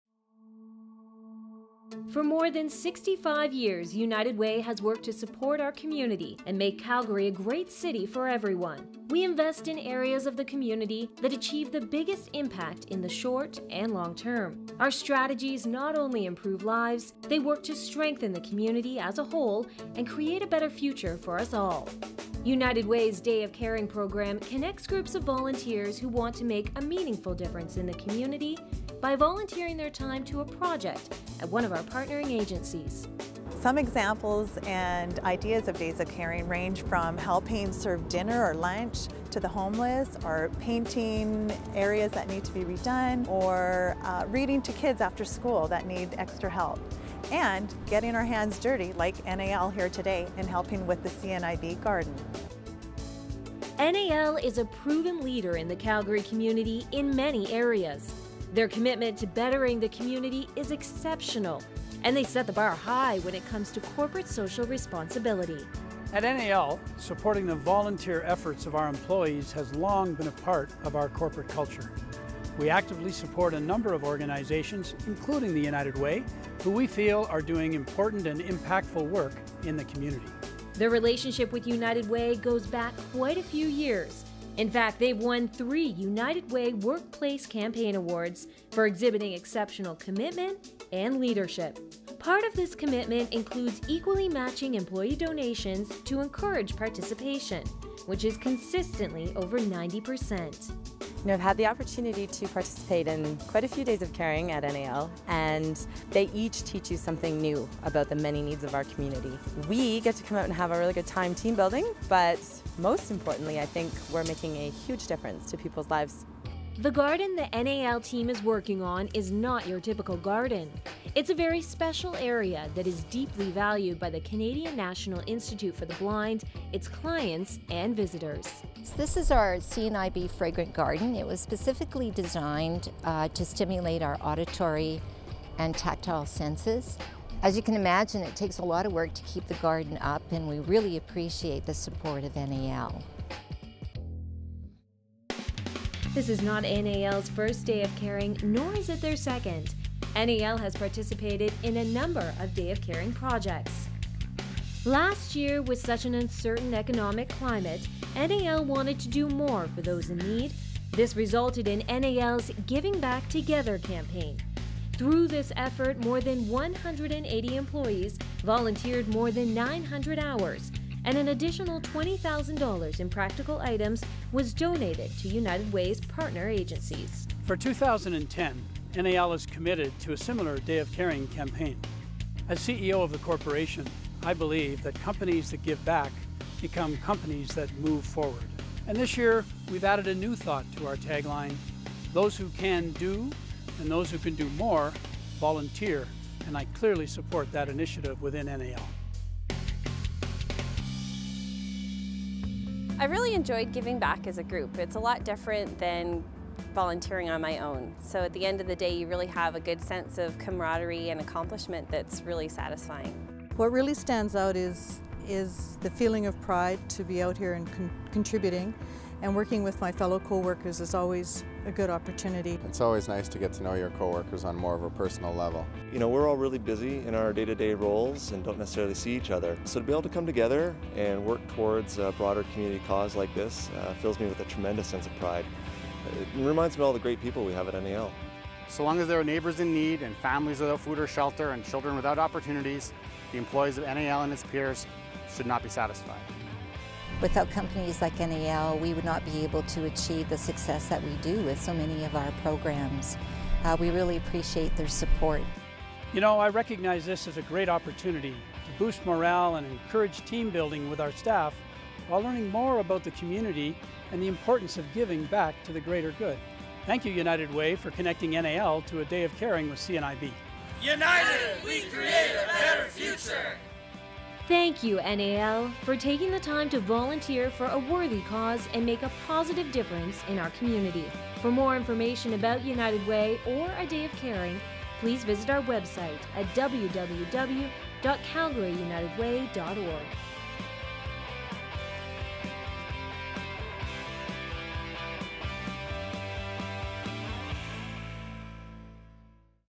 NAL Resources - United Way Day of Caring at CNIB
NAL Resources  is a truly committed community leader in Calgary who have set the bar high when it comes to corporate social responsibility. Check out this video of their latest corporate Day of Caring at CNIB